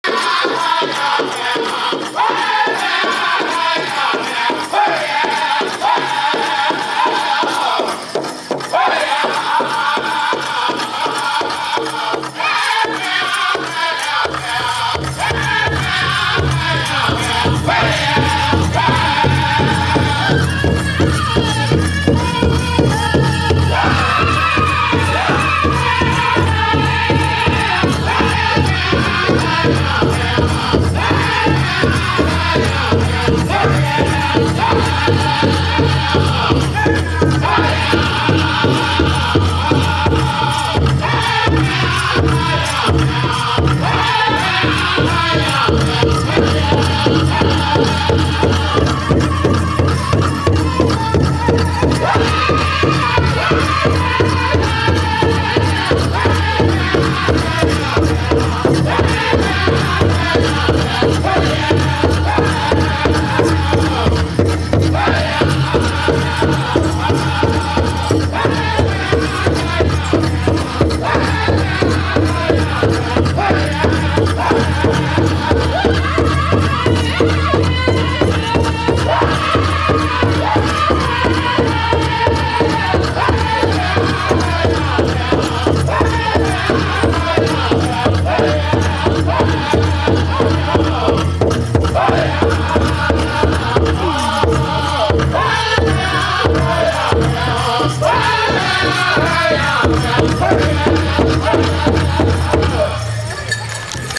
Golden Age Men's Fancy/Grass Sunday Finals in Shakopee August 2015
Sunday Finals at Shakopee Powwow August 14-16, 2015 in Shakopee, Minnesota